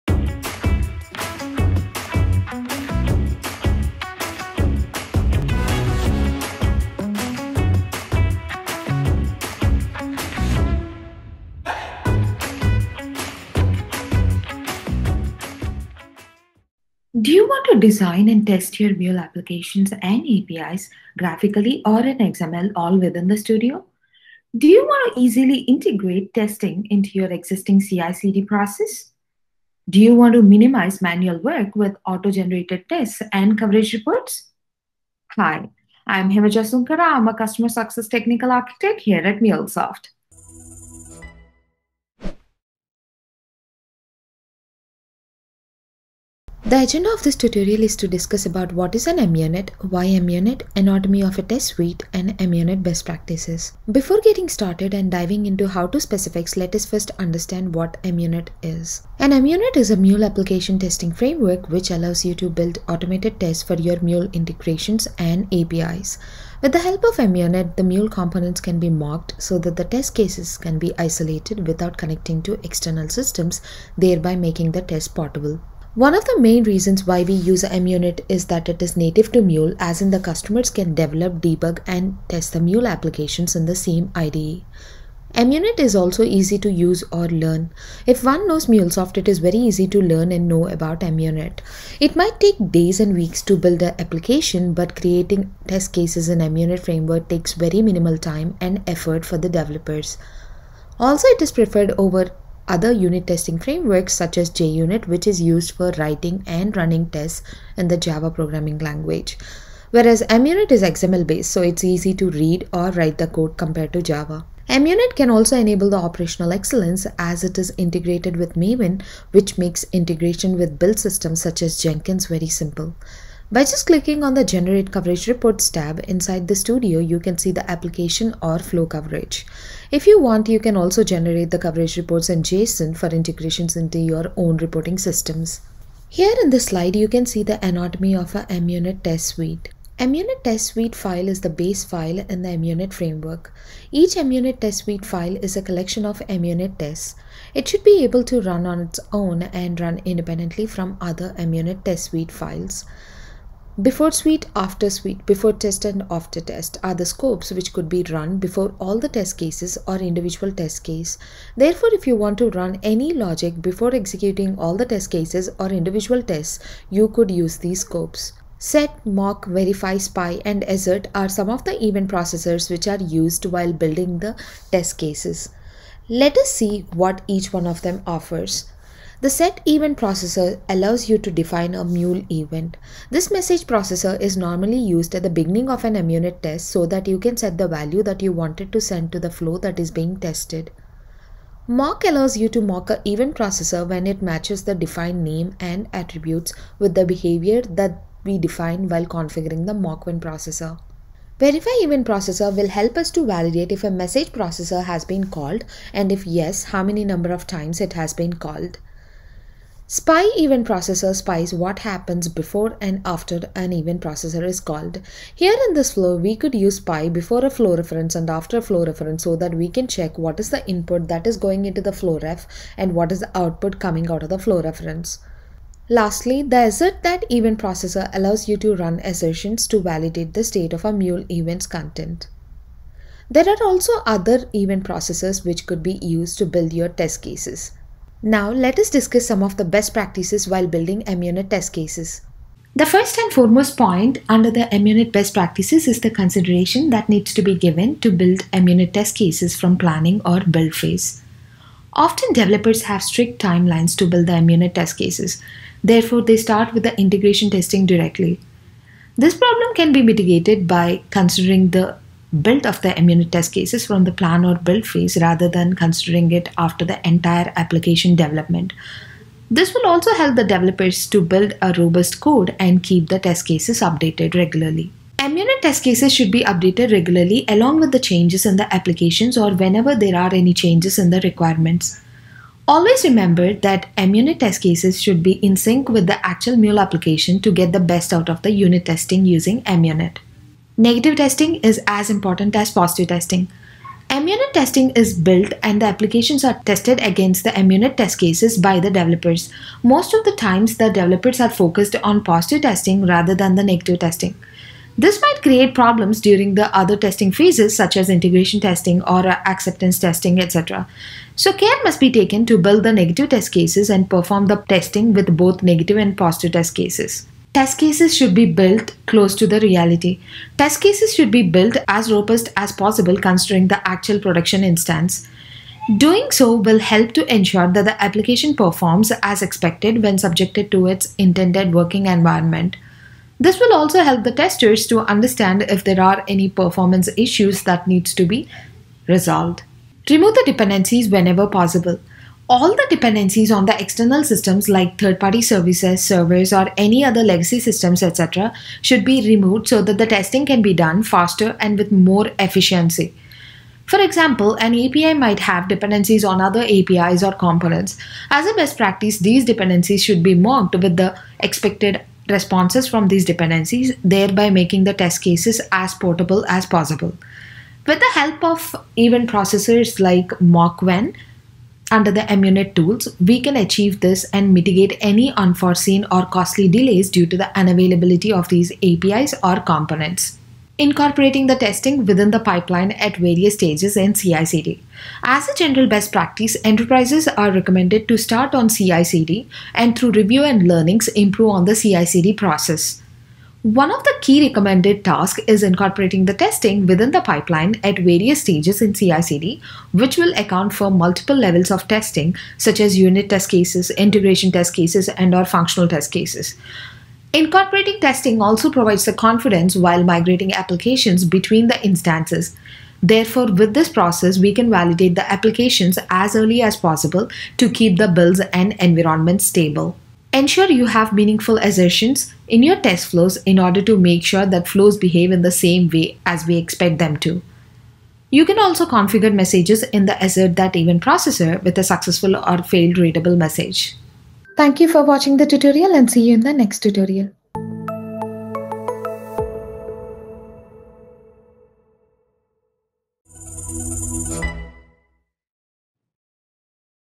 Introduction To Munit | Friends of Max Overview
This video provides an overview of what Munit is, why it is needed, the anatomy of Munit test suite, and best practices to follow.To learn more about this topic, we recommend this free mini course: Failing Fast with MUnit